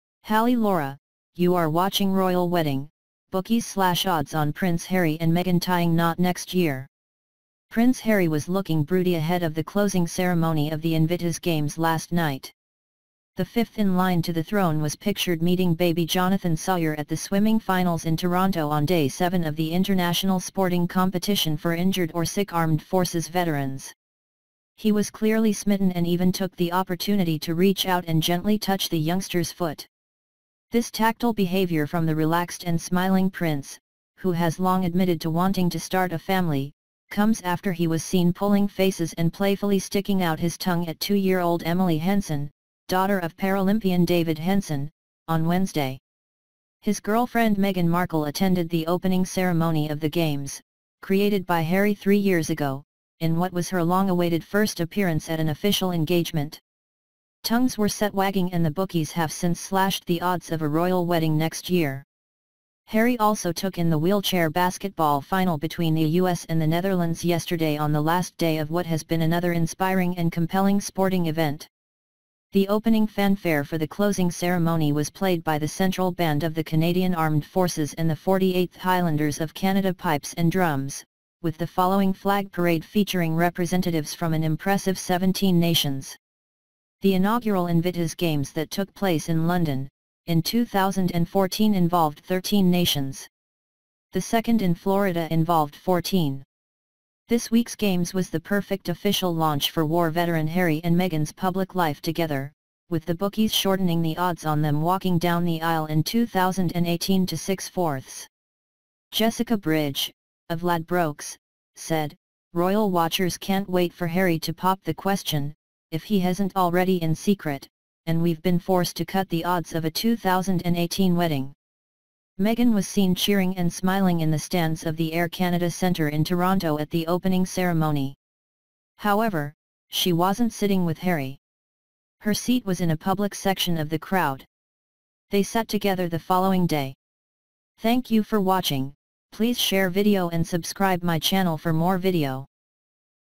NewsRoyal - Royal wedding: Bookies slash odds on Prince Harry and Meghan tying knot next year
Prince Harry was looking broody ahead of the closing ceremony of the Invictus Games last night.